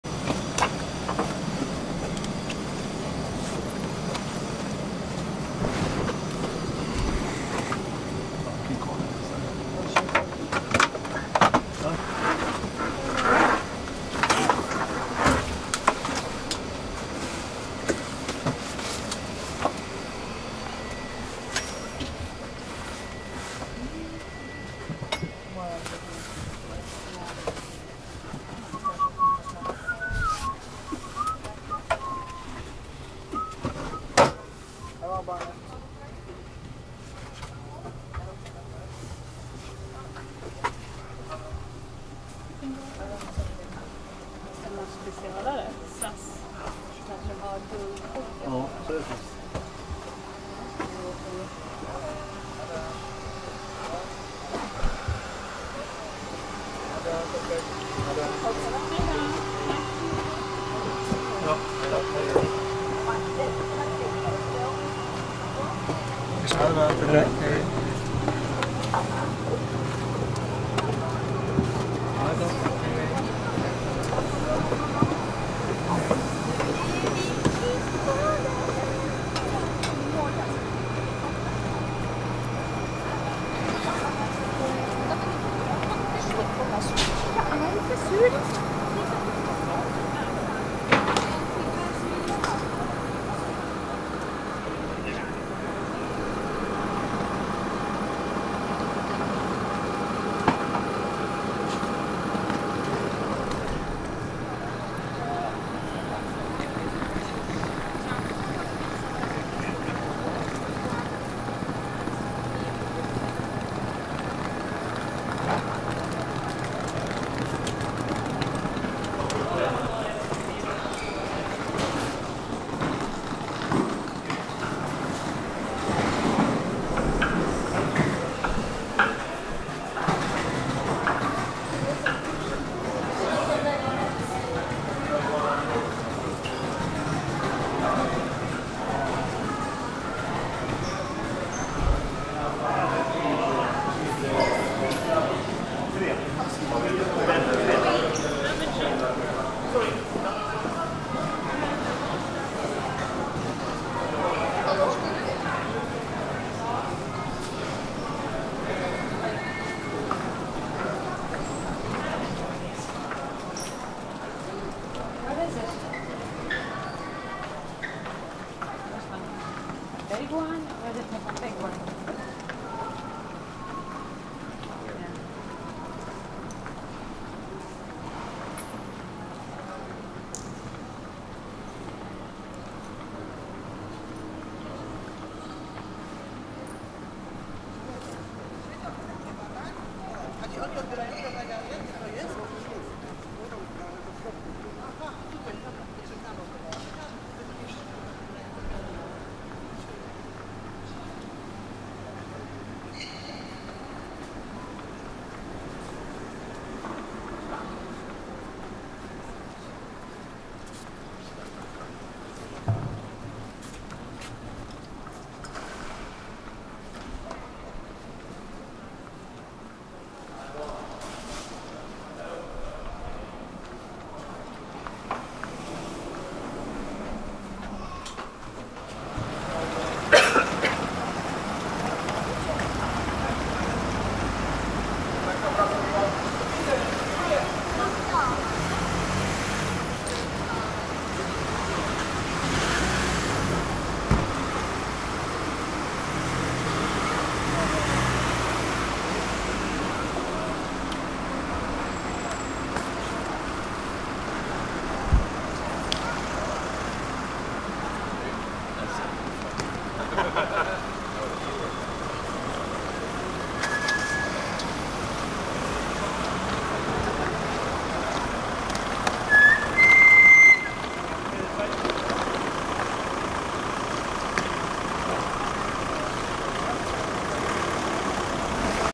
Poland, Gdańsk Lech Wałęsa Airport, Arriving with SAS from Stockholm
July 2018